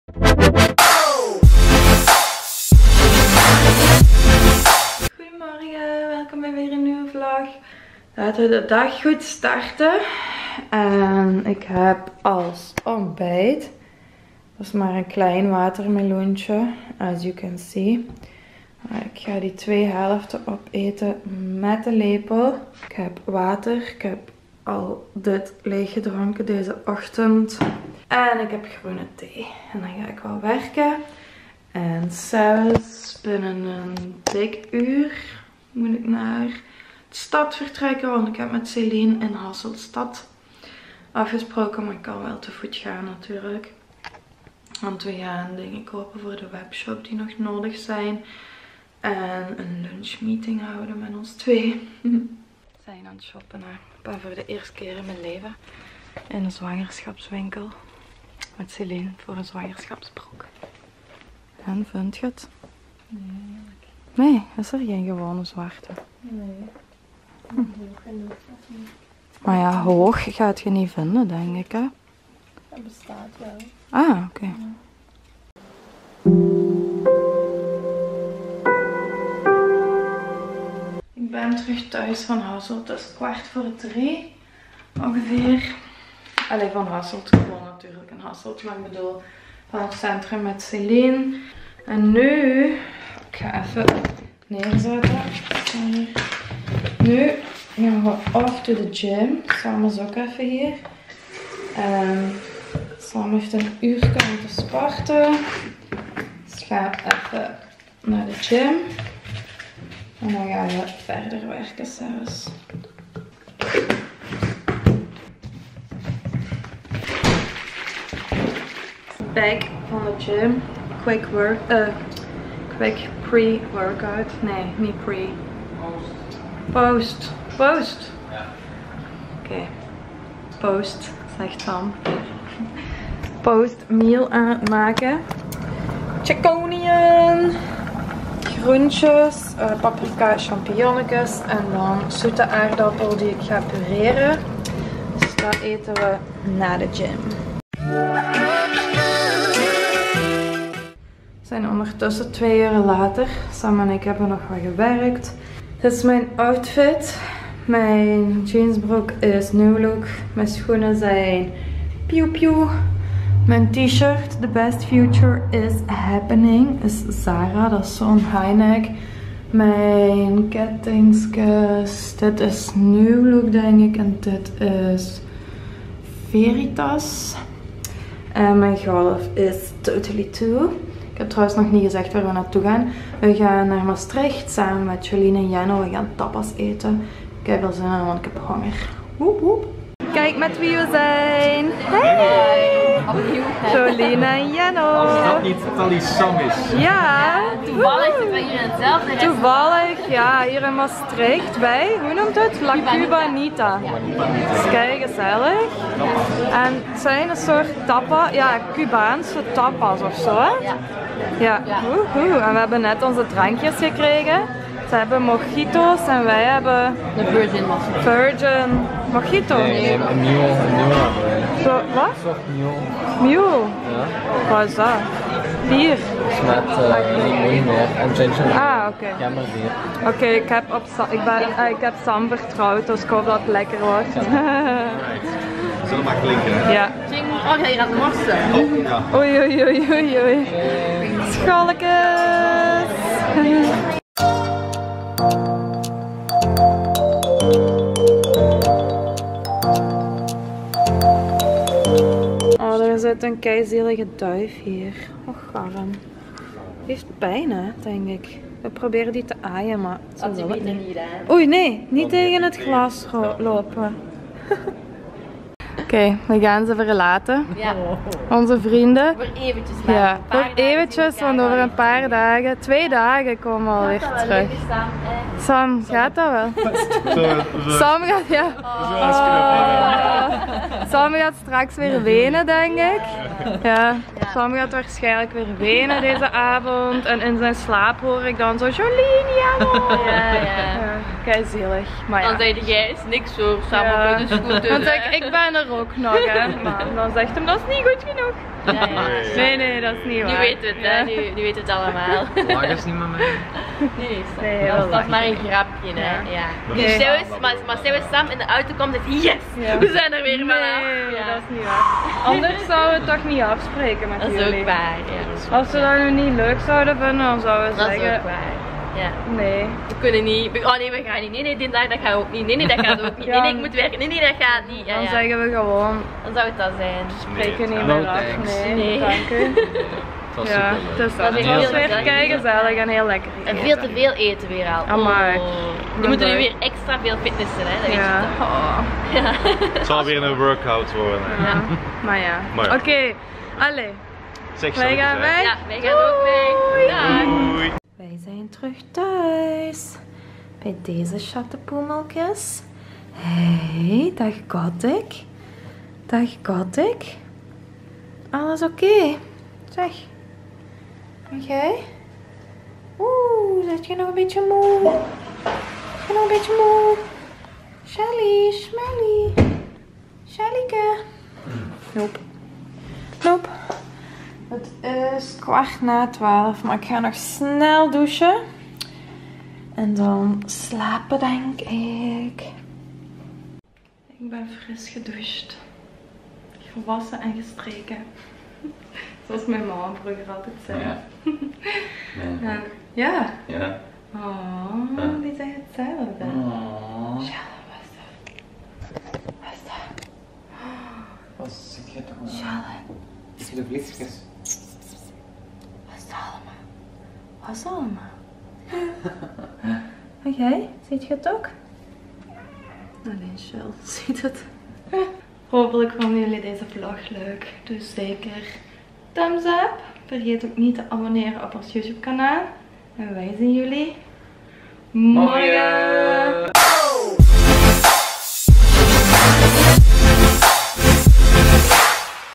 POST GYM FOOD 🍴🍝
Vandaag gaan wij naar de fitness en laten wij onze post fitness maaltijd zien aan jullie. Daarna gaan wij lekker eten met Jolien en Jenno.

Talisam TV